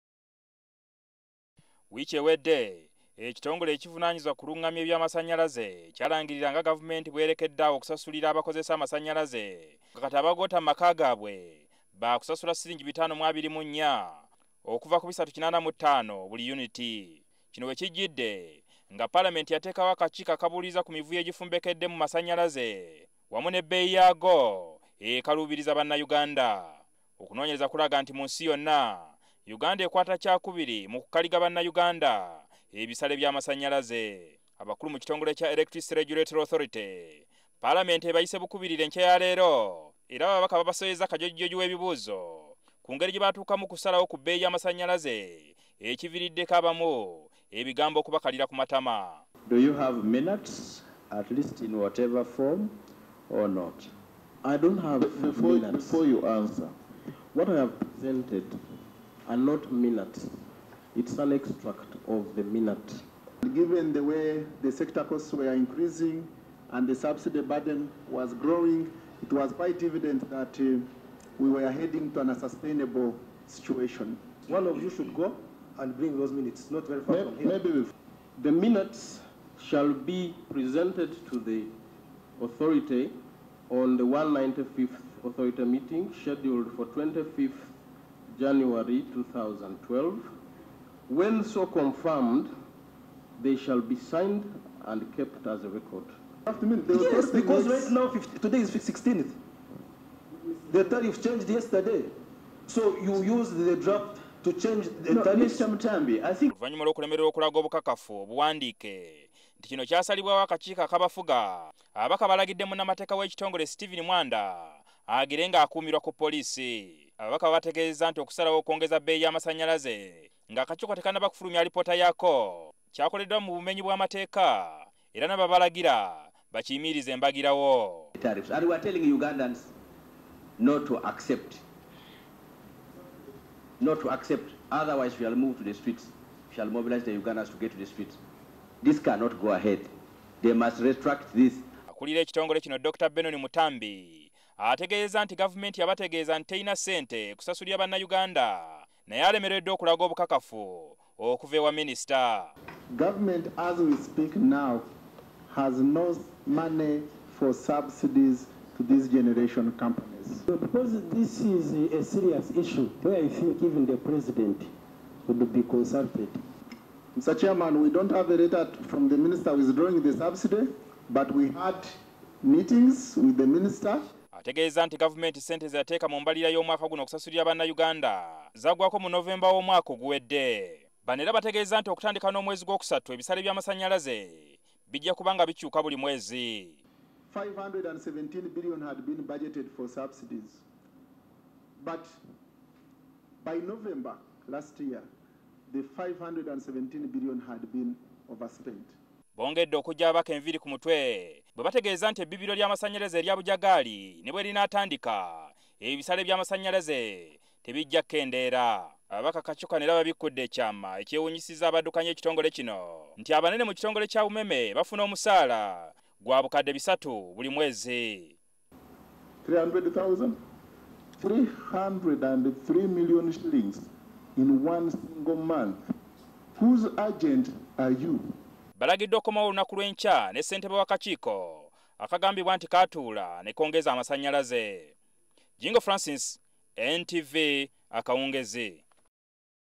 Palamenti ekunyizza ab'amasanyalaze
Ababaka ku kakiiko ka palamenti akabuuliriza ku masanyalaze olwaleero batabukidde ab'ekitongole ekirung'amya ensasaanya ki Electricity Regulatory Authority nga baagala babanyonyole lwaki bayongeza bbeeyi y'amasanyalaze ng'ate akakiiko kakyabuuliriza ebiwerako ebikwata ku masanyalaze. Ababaka balagidde bbeeyi y'amasanyalaze eddeyo nga bweyali okutuusa nga bamaze okukola alipoota yabwe. Jjukira nti bbeeyi empya yatandise okukola ku ntandikwa ya wiiki eno gyetulimu.